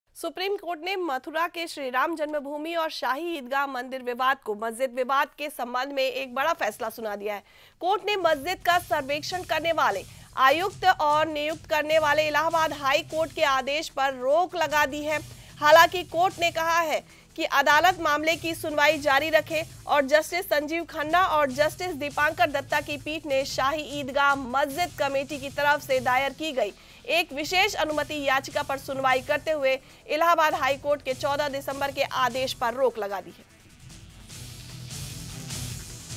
सुप्रीम कोर्ट का बड़ा फैसला….श्रीकृष्ण जन्मभूमि-शाही ईदगाह मस्जिद विवाद मामला….
सुप्रीम कोर्ट का बड़ा फैसला….श्रीकृष्ण जन्मभूमि-शाही ईदगाह मस्जिद विवाद मामला…..जन्मभूमि-मस्जिद विवाद में सर्वे पर लगी रोक….इलाहाबाद हाईकोर्ट के आदेश पर रोक……हाईकोर्ट के आदेश पर रोक लगाने का अंतरिम आदेश….